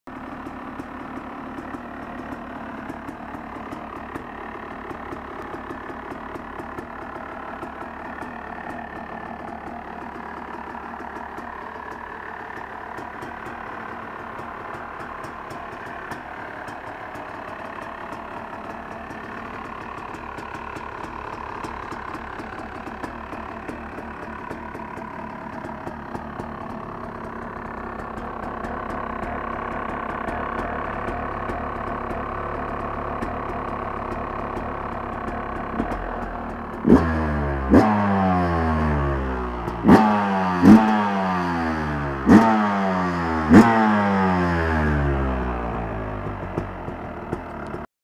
Yamaha YZ250 first start
Yamaha YZ250 2009
Modification works are still in progress :)
Shooted with SJCAM SJ5000 camera